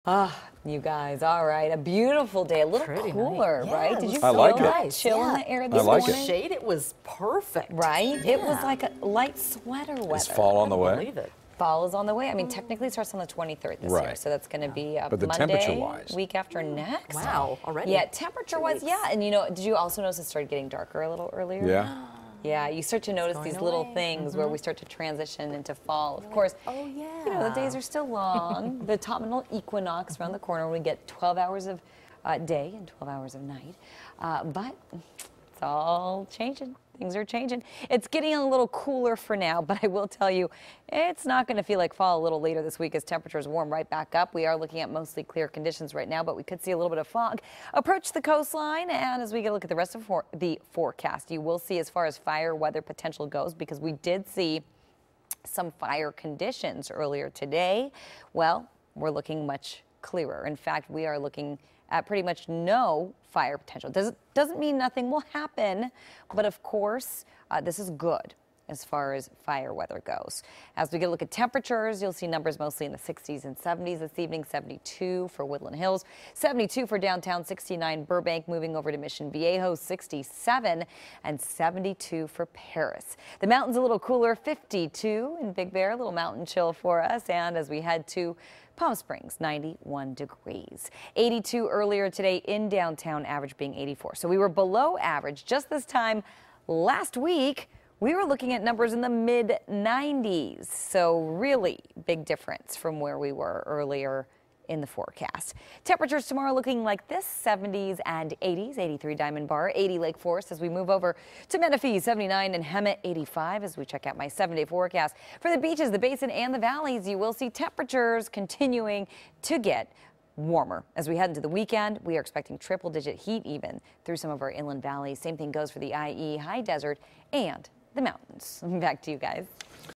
Evelyn Taft's Weather Forecast (Sept. 9)
Evelyn Taft takes a look at tonight's weather forecast.